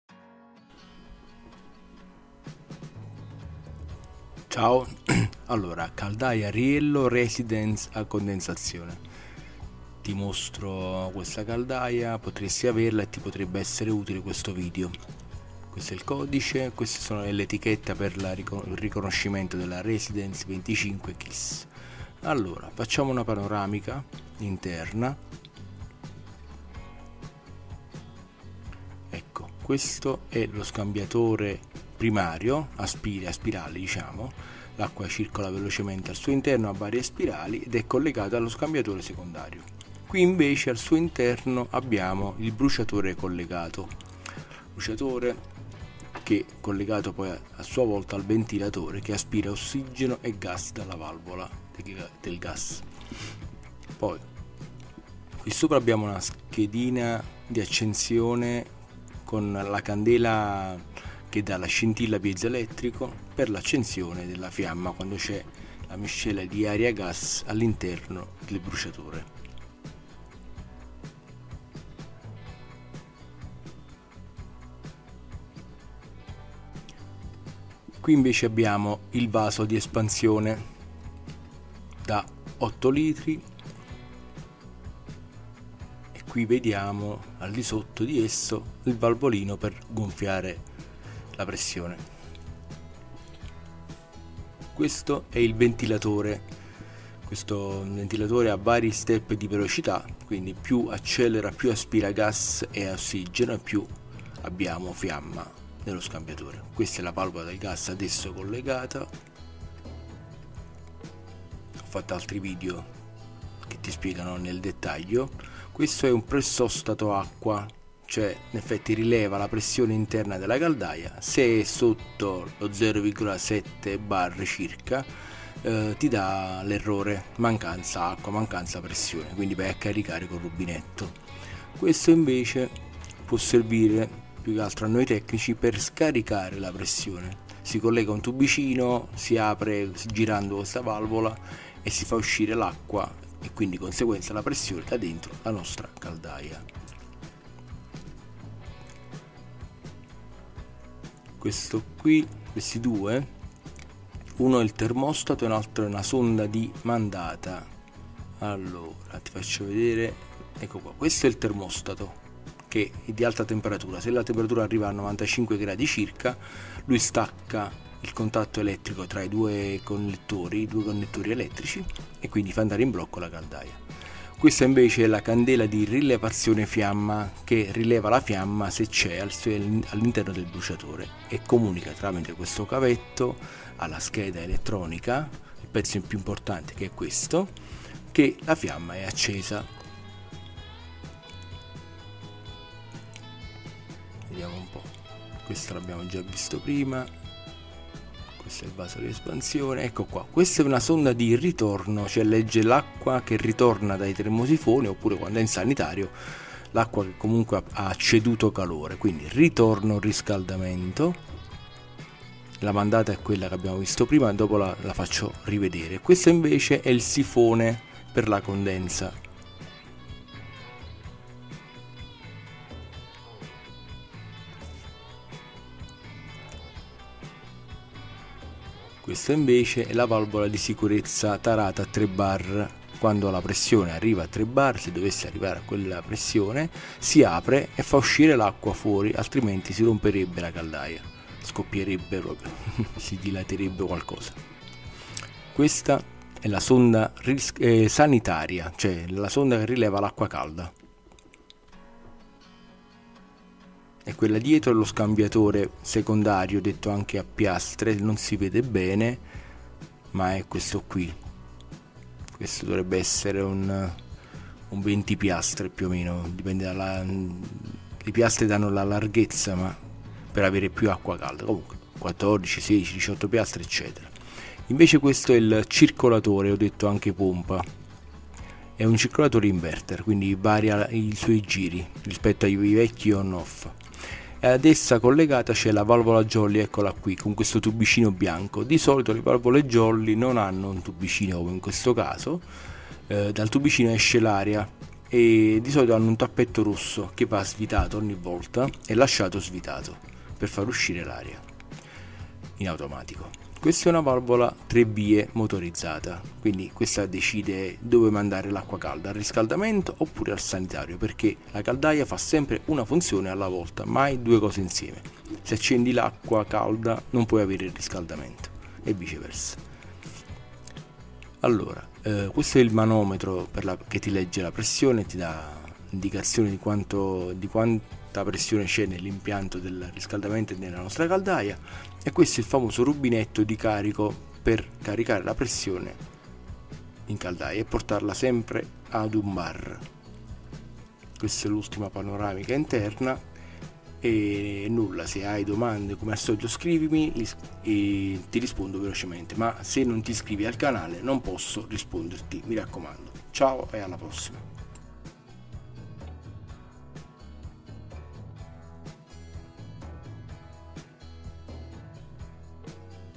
CALDAIA RIELLO RESIDENCE KIS DA QUALI COMPONENTI E' COMPOSTA recensione caldaia
Questa e' una serie di video dedicata alle caldaie a condensazione e i suoi componenti
in questo video n° 1 ti mostro la Riello Residence kis E COME CARICARE LA PRESSIONE

Time line:
00:29 scambiatore primario caldaia
00:43 bruciatore caldaia
00:57 scheda d'accensione e candela d'accensione
01:20 vaso d'espansione caldaia
01:36 ventilatore caldaia
01:49 valvola del gas caldaia
01:57 pressostato acqua caldaia
02:16 valvola per scaricare la pressione in caldaia
02:35 sonda ntc e termostato caldaia
03:00 candela di rilevazione fiamma in caldaia
03:12 scheda elettronica in caldaia
03:32 sifone condensa in caldaia
03:52 valvola di sicurezza 3 bar caldaia
04:10 sonda ntc sanitaria in caldaia
04:20 scambiatore a piastre o secondario in caldaia
04:42 circolatore o pompa in caldaia
04:50 valvola jolly in caldaia
05:06 valvola tre vie motorizzata caldaia
05:25 manometro pressione caldaia
05:34 rubinetto di carico pressione caldaia